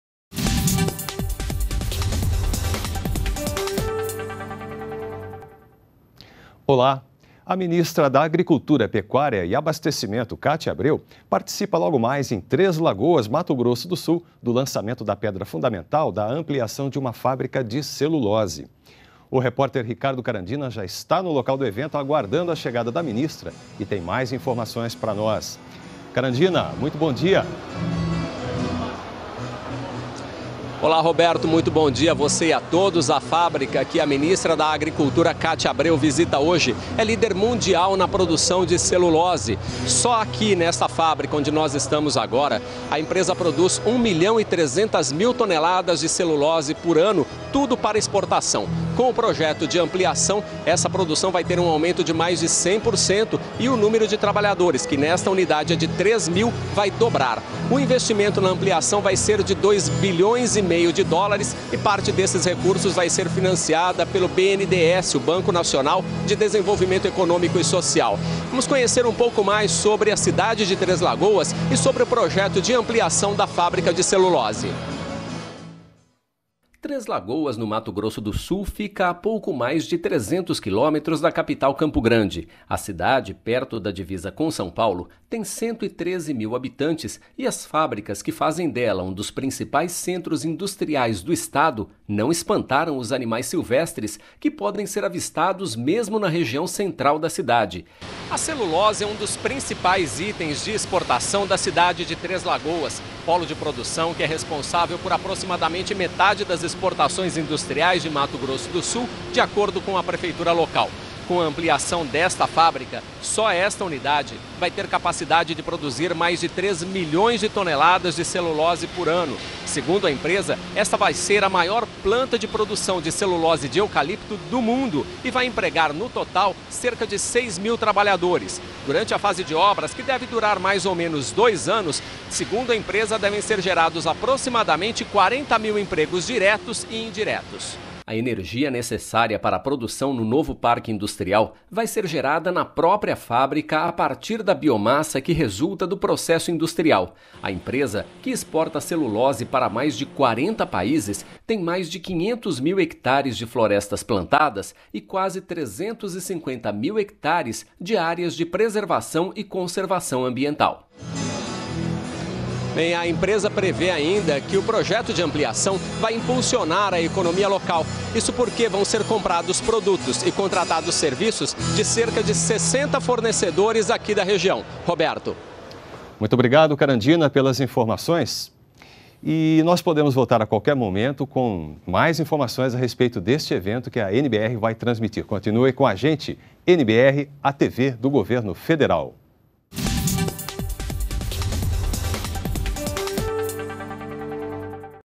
Ampliação de fábrica de celulose vai gerar empregos no Mato Grosso do Sul
ATIVIDADES DO GOVERNO - 30.10.15: Nesta sexta-feira (30), a ministra da Agricultura, Kátia Abreu, participa do lançamento da pedra fundamental de fábrica de celulose em Três Lagoas, no Mato Grosso do Sul.